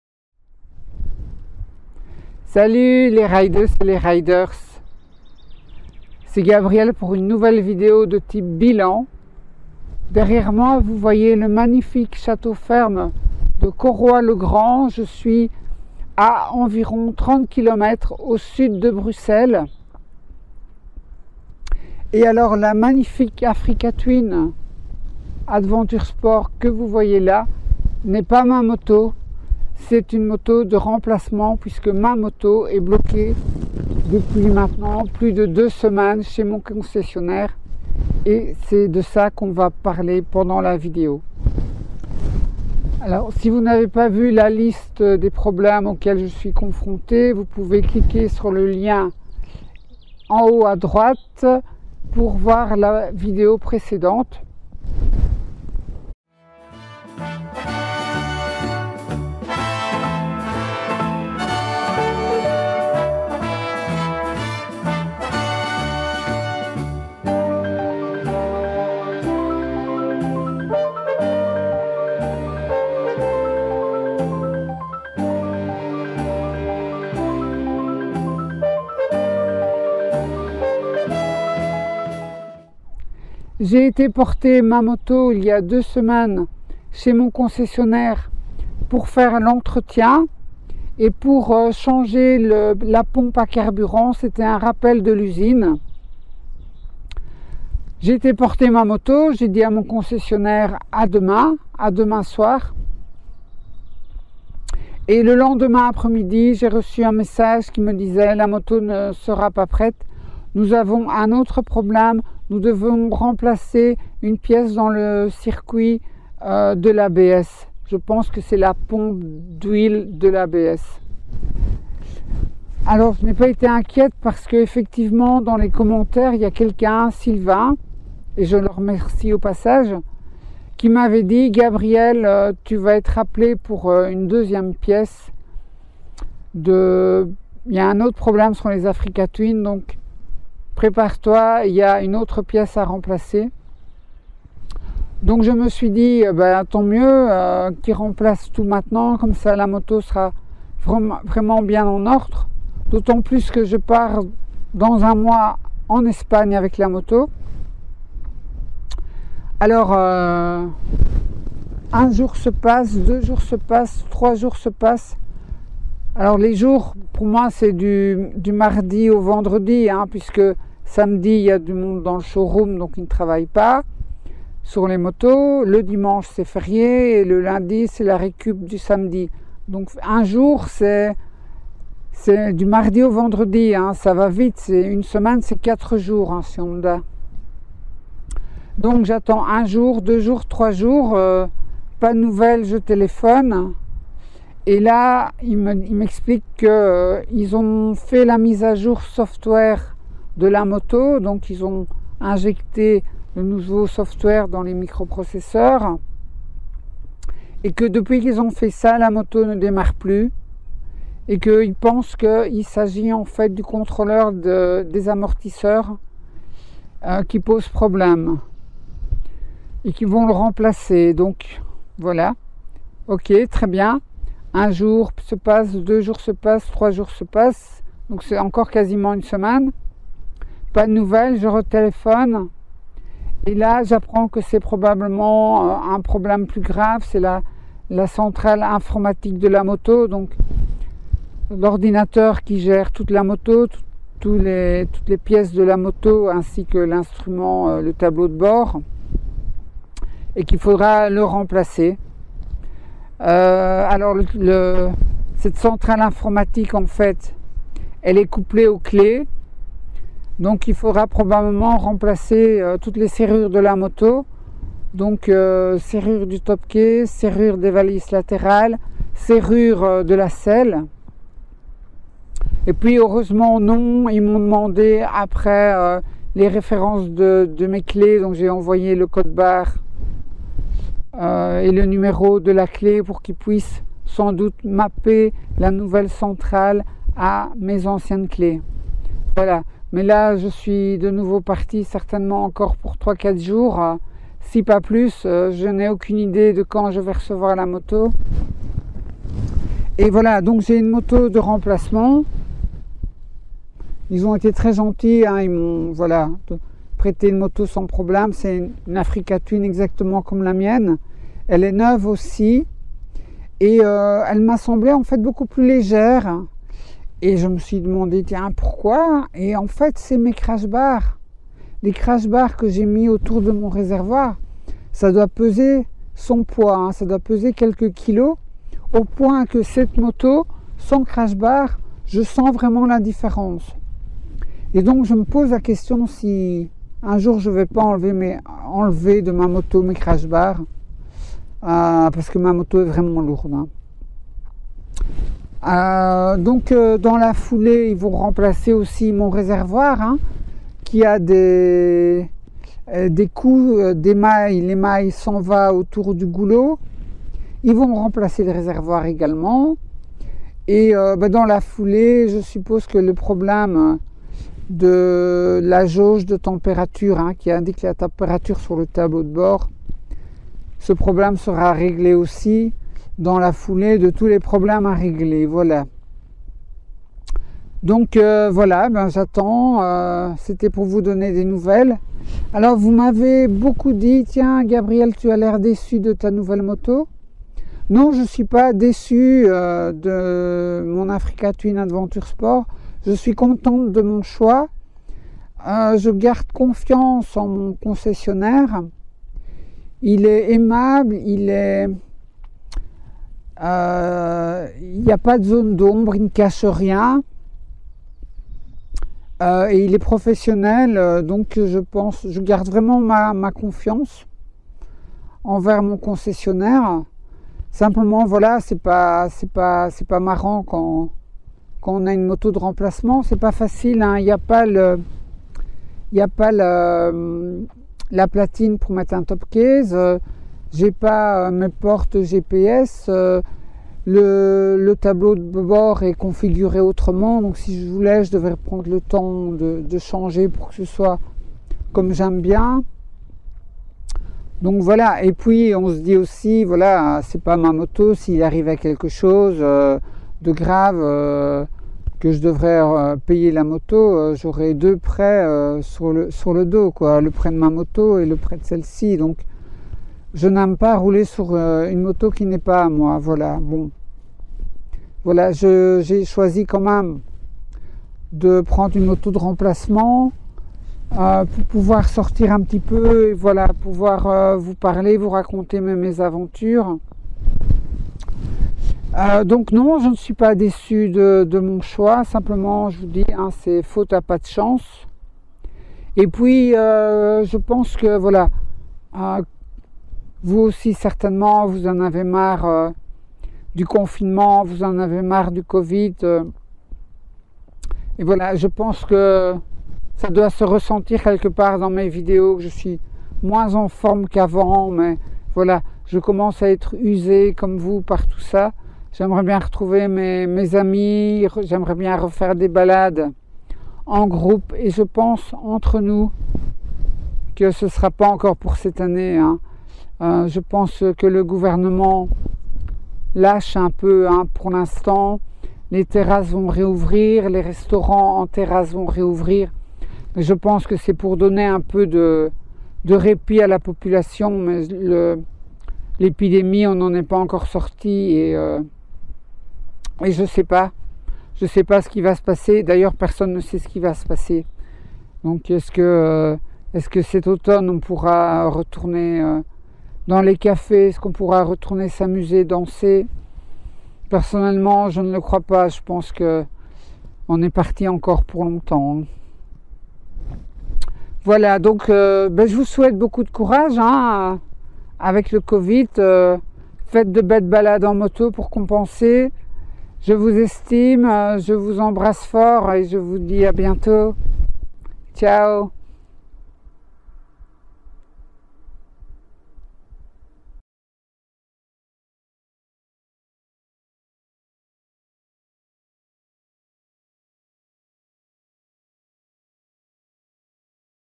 Ma nouvelles ATAS hors-service depuis 2 semaines.
De retour chez le concessionnaire Honda depuis 2 semaines, j'explique les problèmes rencontrés sur ma nouvelle moto.